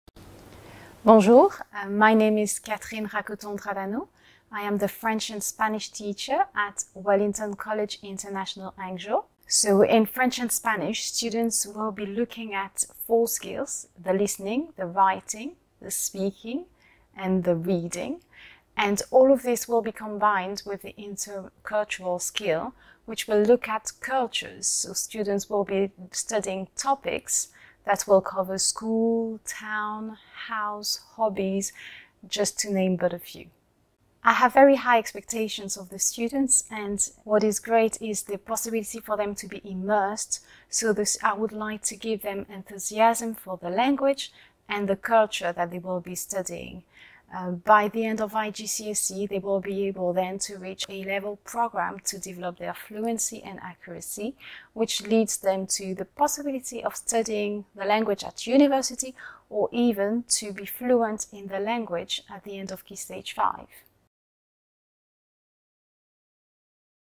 Learning pathway in secondary: French and Spanish
Mastering a foreign language provides an opening to other cultures and fosters a global mindset in young learners. At Wellington College International Hangzhou, we always focus on developing pupils’ confidence and fluency in language acquisition. Starting from this academic year, French and Spanish have been successfully introduced into our curriculum. It is amazing to see our pupils embracing learning a third language with  continuing regular learning in English and Mandarin. When they graduate from the College, it enables our pupils to express their ideas and thoughts confidently and fluently in spoken and written languages. This is also central to our curriculum.

In this video, Ms. Catherine Rakotondradano, French and Spanish teacher, offers a brief introduction about the two additional languages now being offered in the secondary school.